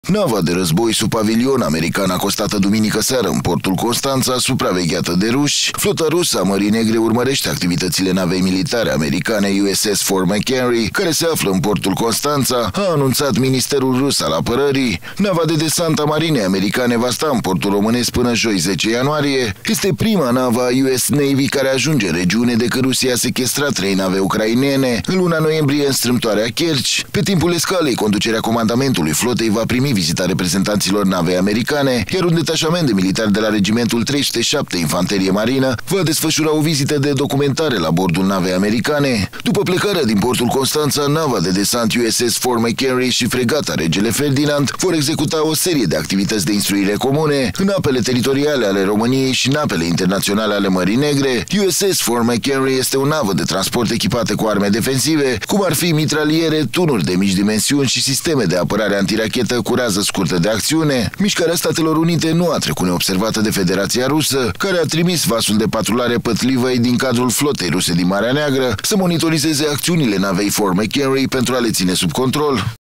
Nava de război americană din Portul Constanța, supravegheată de ruși
Nava de război sub pavilion american acostată, duminică seară, în portul Constanța, supravegheată de ruși! Flota rusă a Mării Negre urmărește activitățile navei militare americane USS Fort McHenry, care se află în portul Constanța, a anunțat Ministerul rus al Apărării. Nava de desant USS Fort McHenry a marinei americane va sta în portul românesc până joi 10 ianuarie. Este prima navă a U.S. Navy care ajunge în regiune, de când Rusia a sechestrat trei nave ucrainene, în luna noiembrie, în strâmtoarea Kerci. Pe timpul escalei, conducerea Comandamentului Flotei va primi vizita reprezentanților navei americane, iar un detașament de militari de la Regimentul 307 Infanterie Marină va desfășura o vizită de documentare la bordul navei americane. După plecarea din Portul Constanța, nava de desant USS Fort McHenry și Fregata "Regele Ferdinand" vor executa o serie de activități de instruire comune, în apele teritoriale ale României și în apele internaționale ale Mării Negre. USS Fort McHenry este o navă de transport echipată cu arme defensive, cum ar fi mitraliere, tunuri de mici dimensiuni și sisteme de apărare antirachetă cu rază scurtă de acțiune. Mișcarea Statelor Unite nu a trecut neobservată de Federația Rusia care a trimis vasul de patrulare Pâtlivâi, din cadrul Flotei ruse din Marea Neagră, să monitorizeze acțiunile navei Fort McHenry, pentru a le ține sub control.